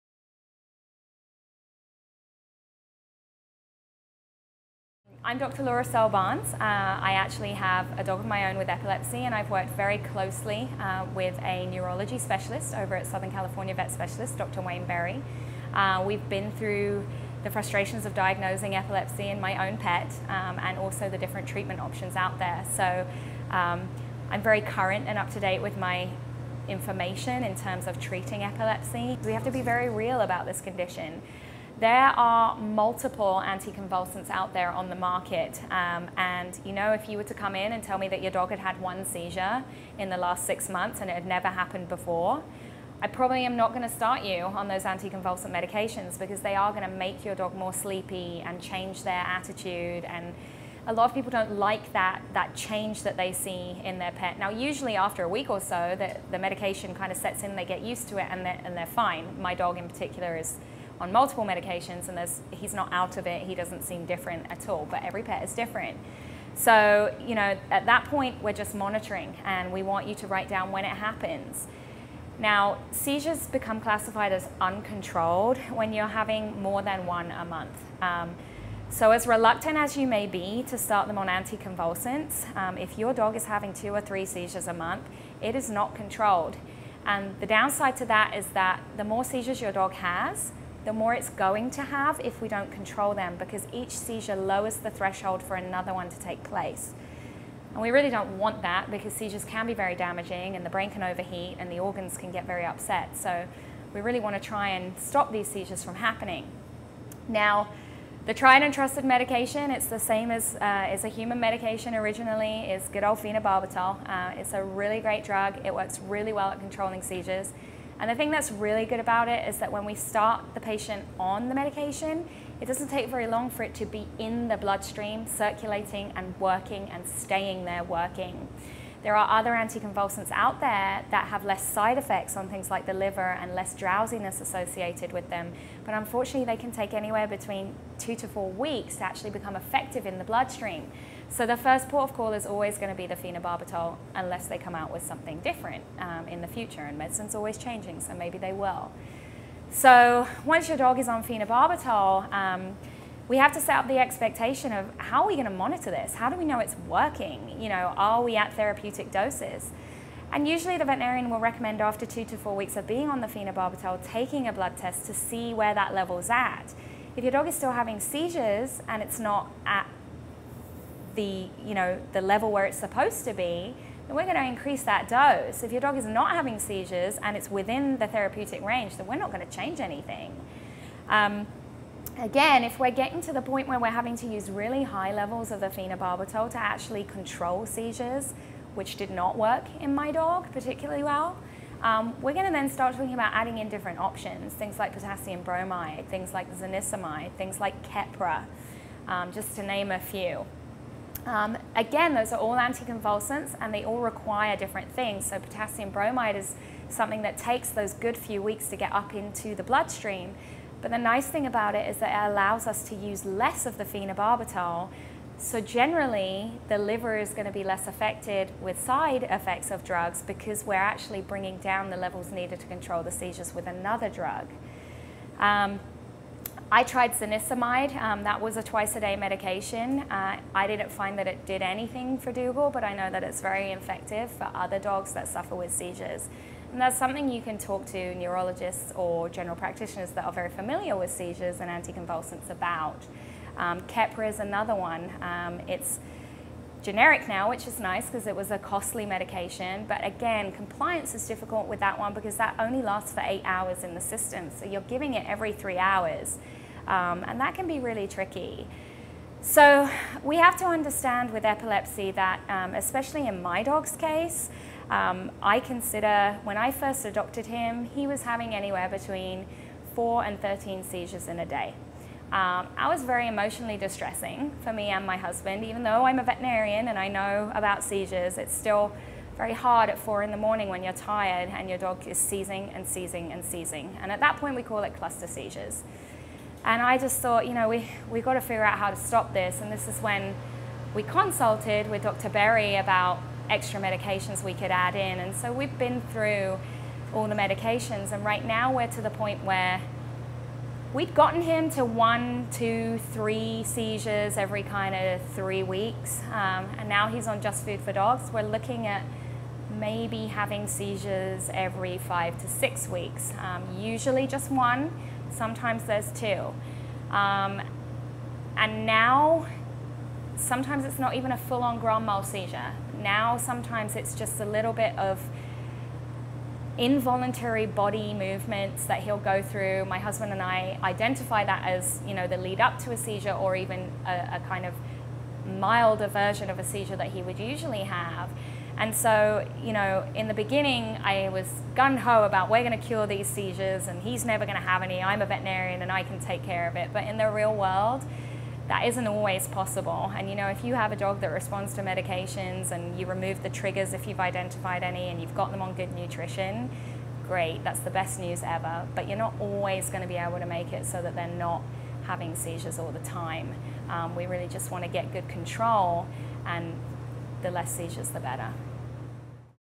Ask The Experts: How Do I Treat A Dog With Epilepsy? | JustFoodForDogs
Dr. Laura Searle-Barnes, pet parent of Dougal, a dog with epilepsy, shares her experience and knowledge on treating this difficult disease.  This is a very personal topic for Dr. Laura, and we are thrilled to have her share her experience with all of us.